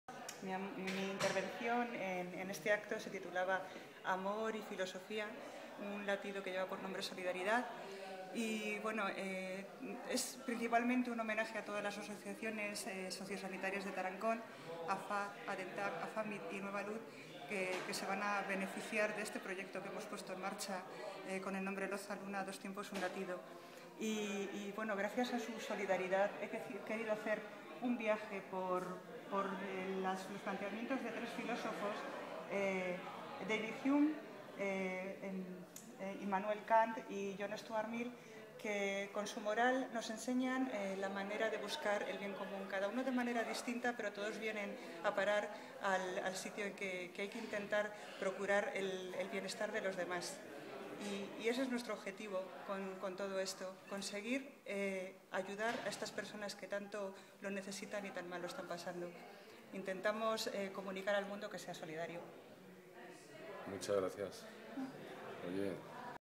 "Amor y filosofía, un latido que lleva por nombre solidaridad"
Milagros Carranza ha hablado de "Amor y filosofía , un latido que lleva por nombre SOLIDARIDAD". Una charla  organizada por el proyecto solidario "Loza, Luna, dos tiempos...,un latido", en Tarancón (Cuenca), con el fin de recaudar y ayudar económicamente a las asociaciones asistenciales del pueblo.